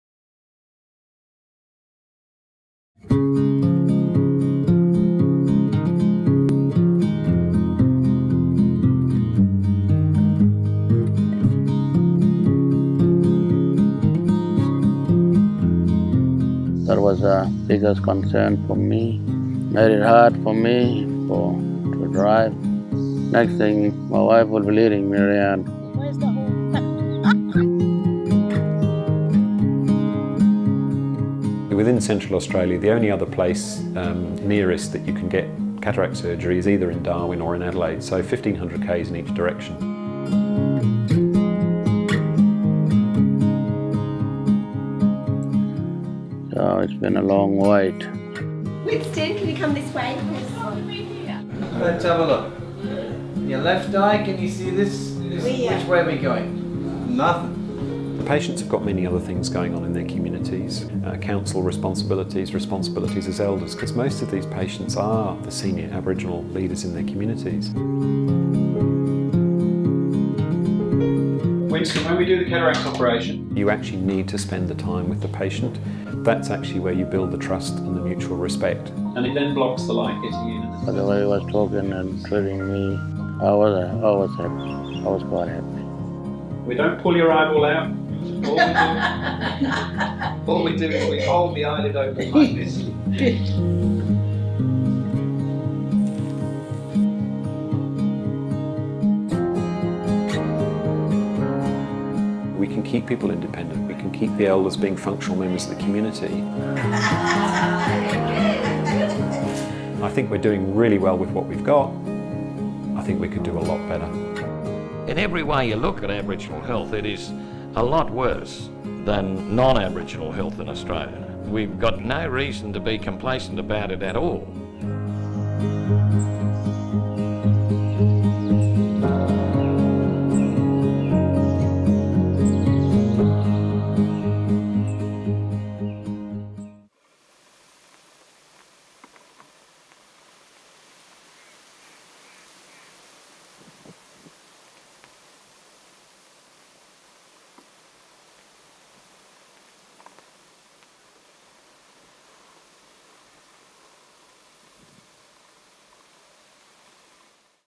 Australians like Winston don't need to be blind
Winston is an Aboriginal Australian who works as a traditional land manager and camel herder. But his deteriorating vision due to cataract threatened his ability to work.

Aboriginal and Torres Strait Islander people are three times more likely to go blind than other Australians.

Winston received treatment at the Alice Springs Intensive Eye Surgery Week with support from The Fred Hollows Foundation and its partners (CABIEHS). This is an important service for communities across Central Australia.

Ngaanyatjarra Camel Company footage courtesy of NG Media
Drone footage of Central Australia courtesy of Johan Dahlberg